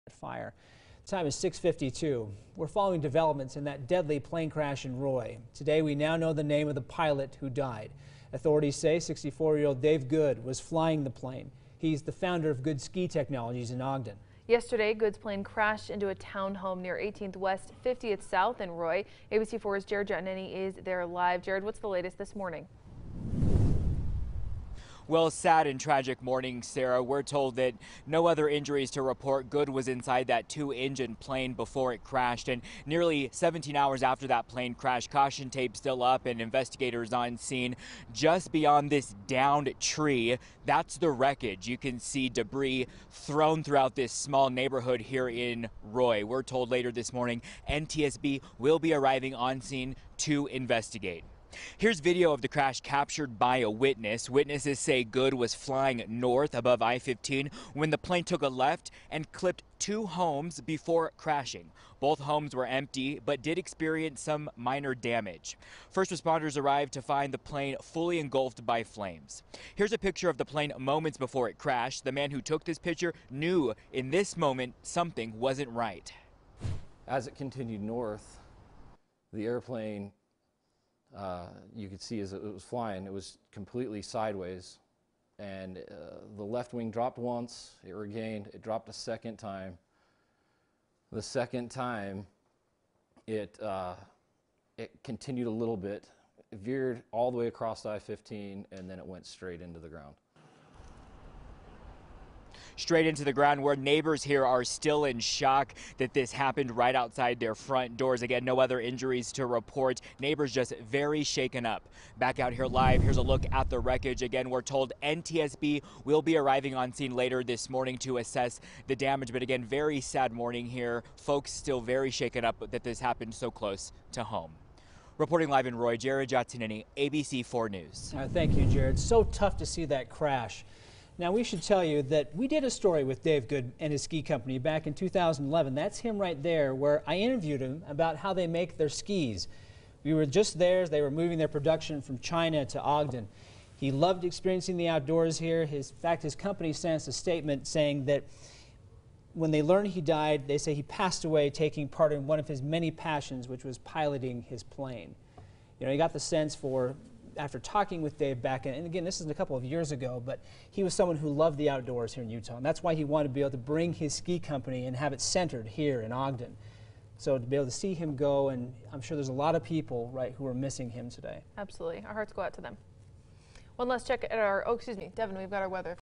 Roy Fatal Plane Crash
Dave Goode was piloting the plane. We spoke to him a few years ago about his ski company.